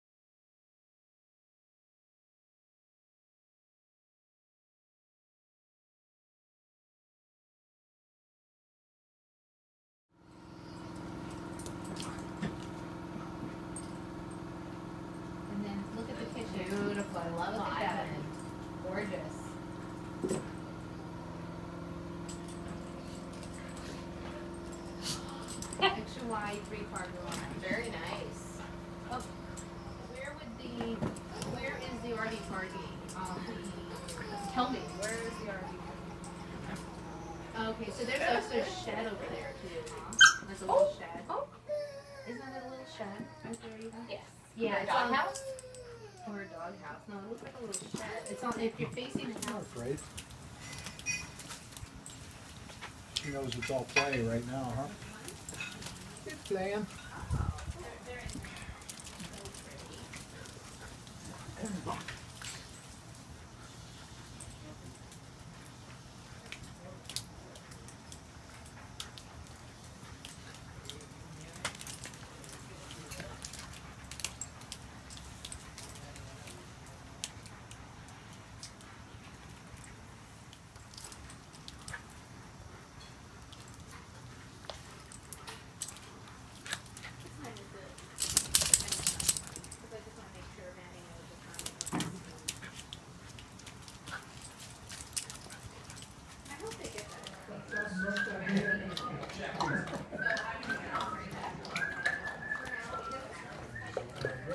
Fran's trip to hang with Max for Annie's bday 2016
Francine made the trip to PQ for Annie's bday on 3/6/16. They got along swimmingly.